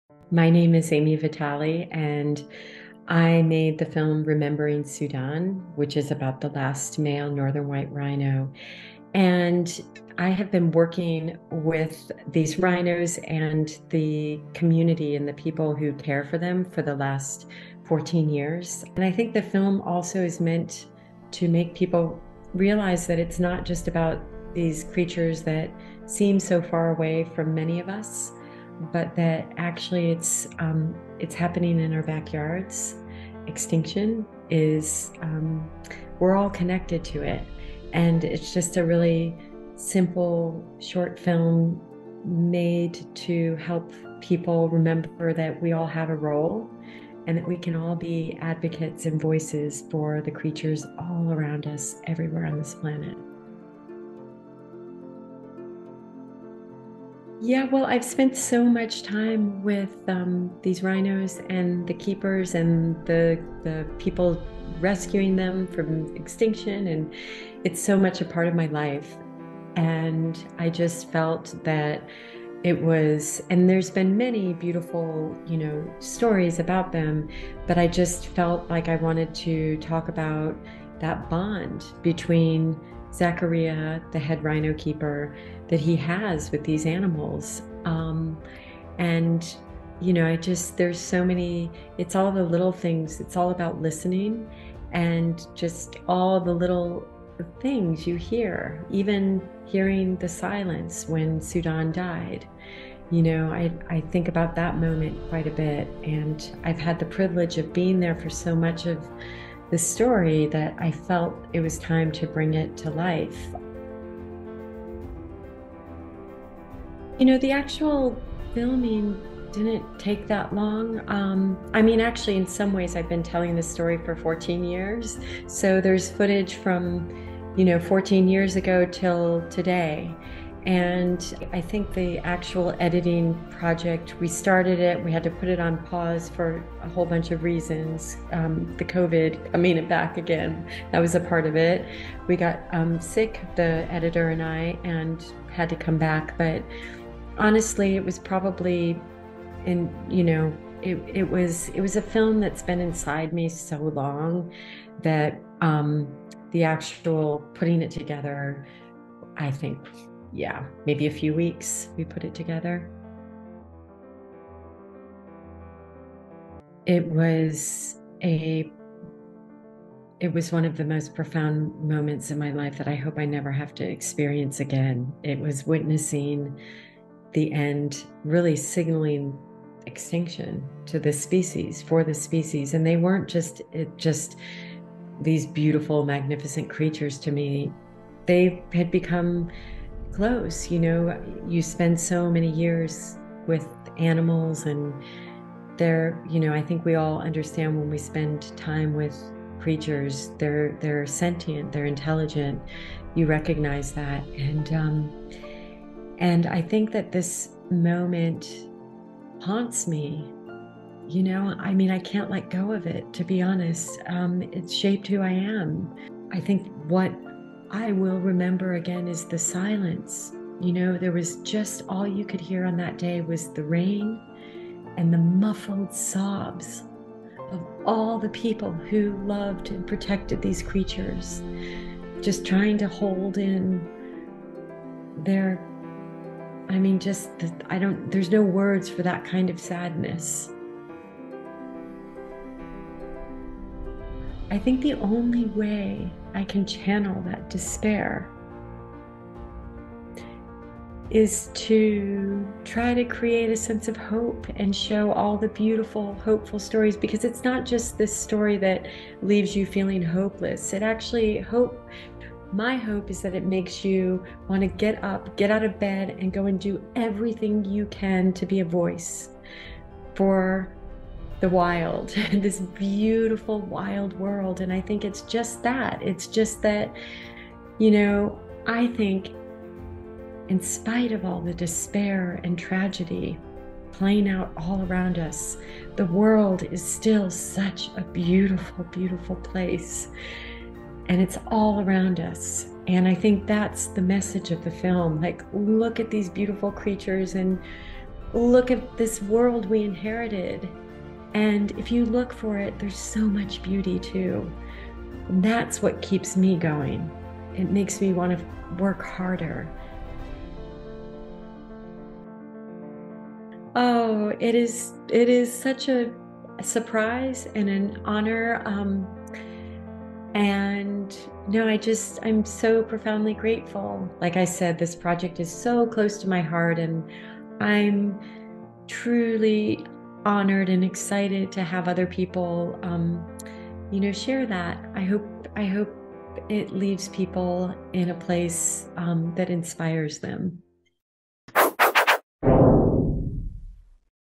ÉCU - INTERVIEW Ami Vitale
Interview with Ami Vitale about her film REMEMBERING SUDAN